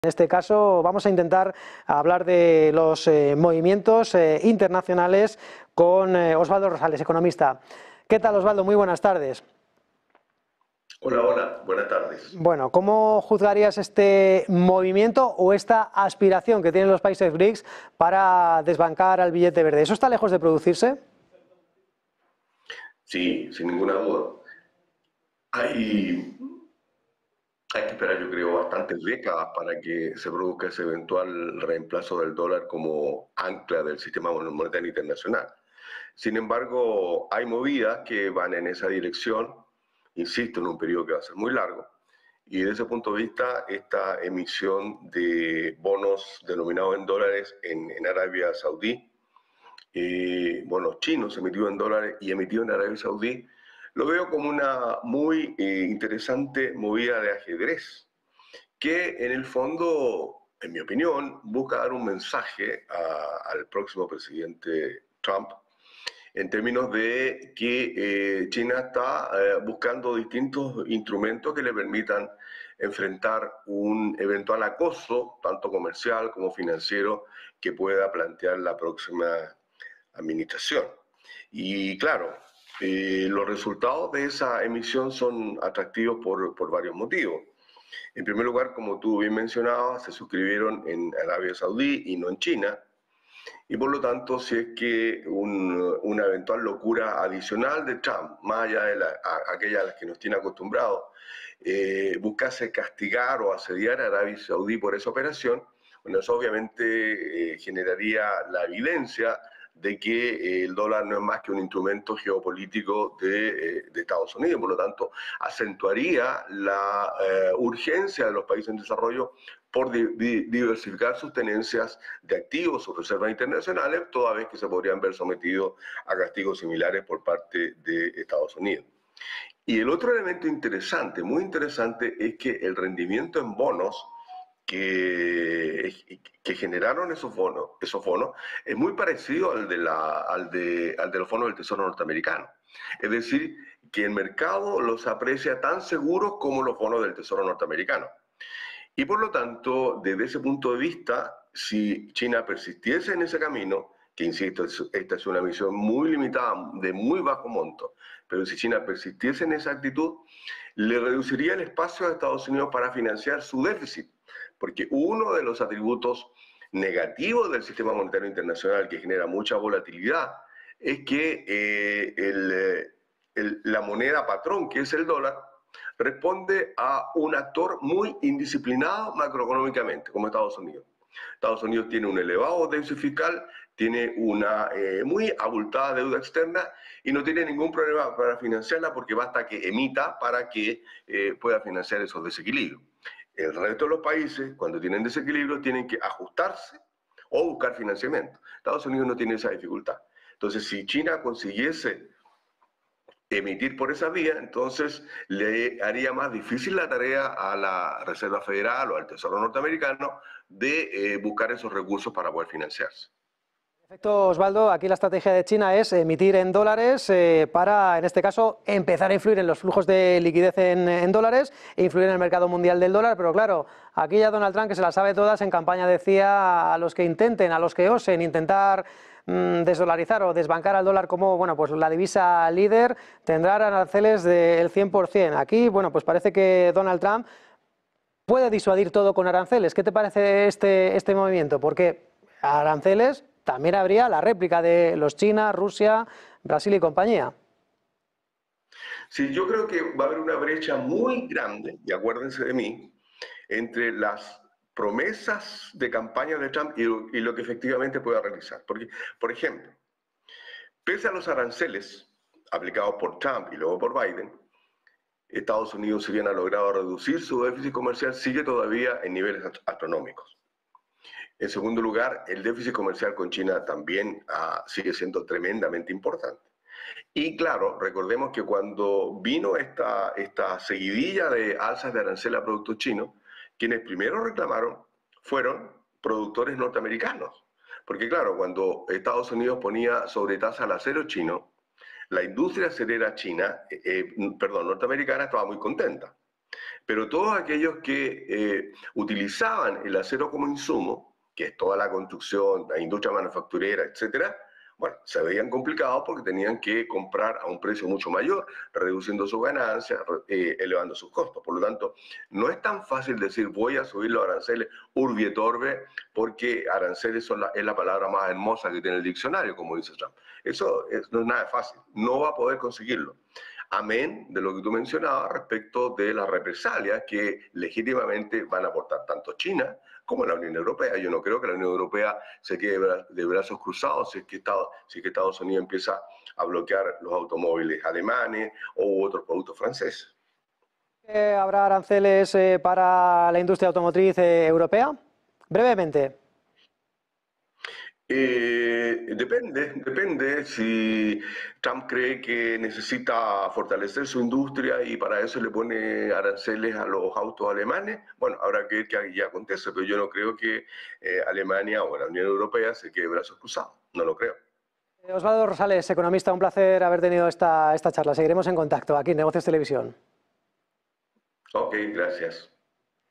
"China está emitiendo bonos en dólares con tanta demanda como para acabar con la influencia de EEUU"
Osvaldo Rosales, economista, habla sobre los esfuerzos de los países BRICS para desbancar al dólar como moneda de reserva mundial. Rosales considera que este cambio está muy lejos de materializarse, posiblemente en varias décadas, aunque reconoce movimientos en esa dirección. Un ejemplo de estos es la emisión de bonos denominados en dólares por parte de Arabia Saudita, emitidos en colaboración con China, lo que podría ser una jugada estratégica para enfrentar posibles políticas de acoso comercial y financiero bajo la próxima administración estadounidense. Rosales también destaca que los bonos emitidos en este contexto fueron muy bien valorados por el mercado, al igual que los bonos del tesoro estadounidense, lo que podría reducir la capacidad de EE. UU. de financiar su déficit.

Respecto a la amenaza del presidente electo de EEUU, Donald Trump, de imponer aranceles a los países que intenten desdolarizar, Rosales cree que aunque las promesas de campaña son contundentes, implementar estos aranceles sería complicado debido a las implicaciones económicas para los propios productores estadounidenses, quienes también se verían afectados por el alza de precios en los insumos provenientes de China. Además, no descarta represalias por parte de China y la Unión Europea, lo que podría generar una guerra comercial difícil de manejar.